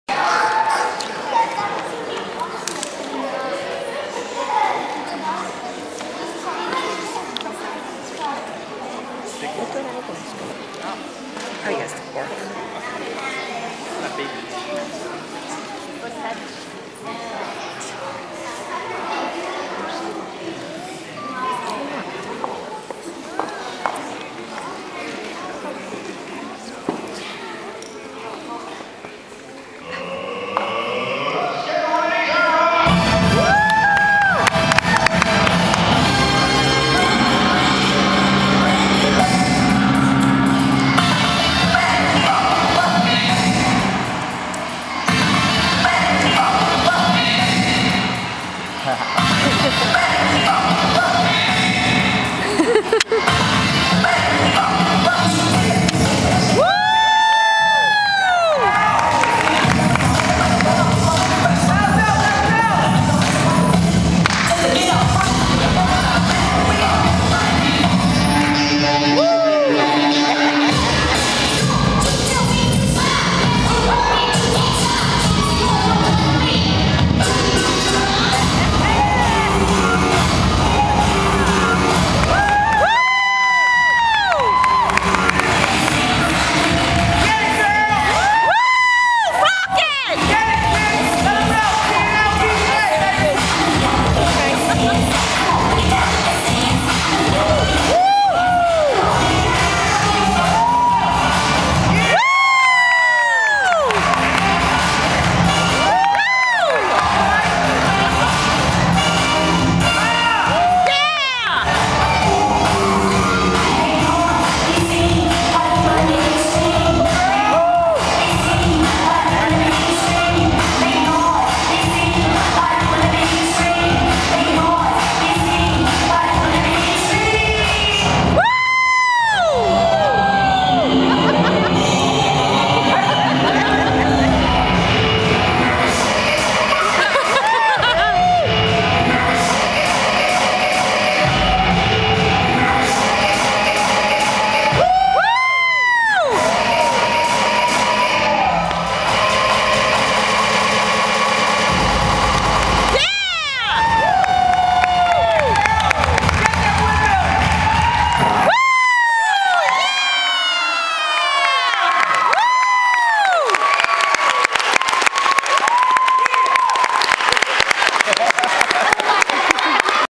036
B girl Kick from Black Belt Academy Rocks it at Saville Talent Show. Props to her instructor's Frank Logic & B-boy Ivan